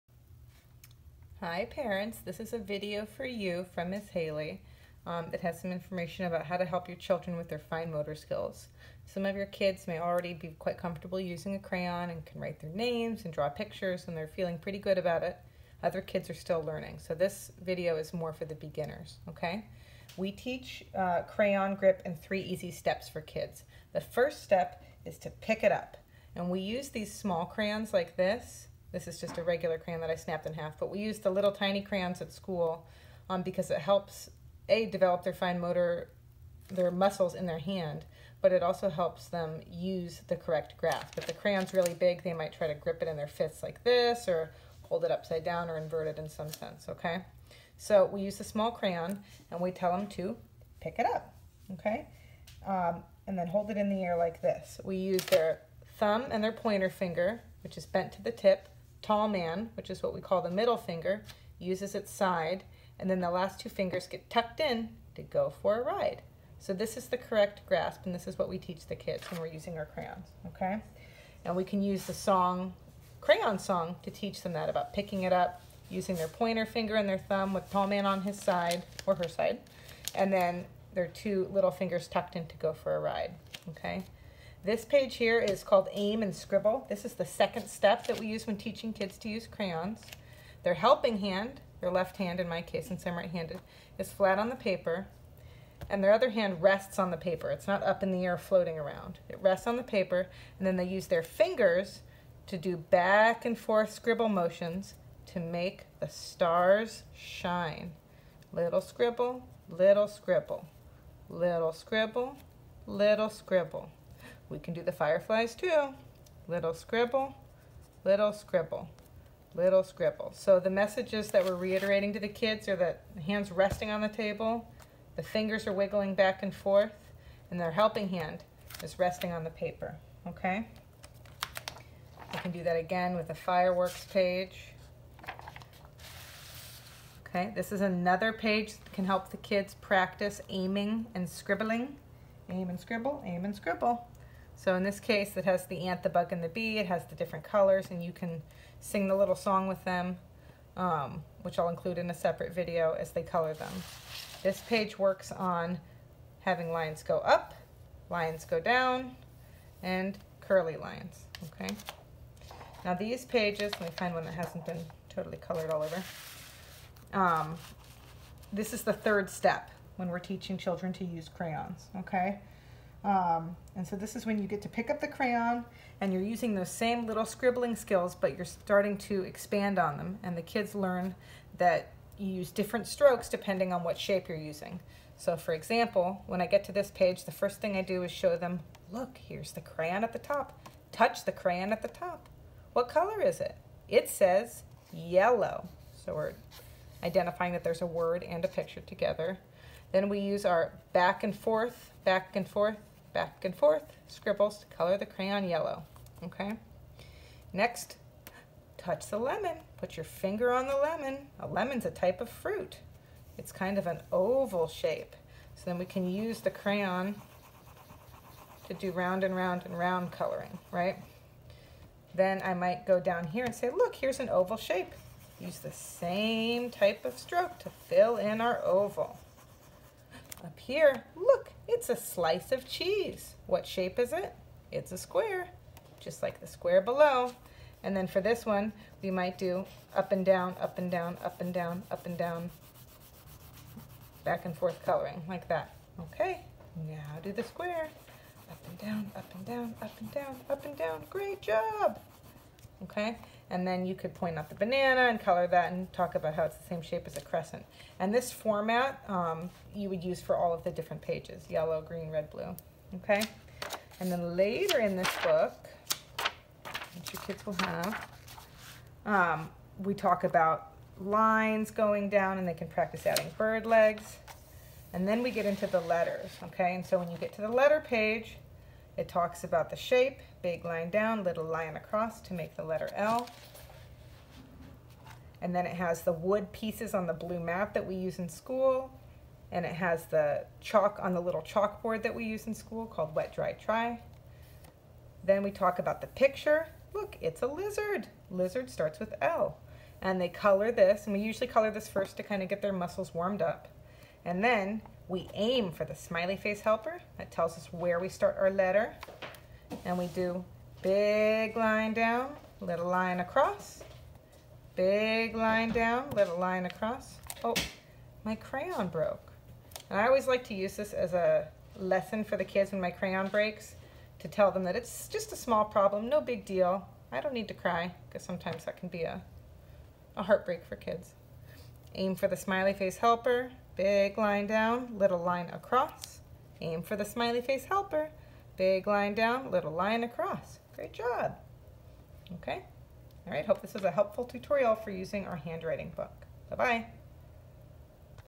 Crayon Grip / Aim & Scribble
A brief introduction to some of the language and strategies we use in the classroom when teaching children how to hold and use a crayon.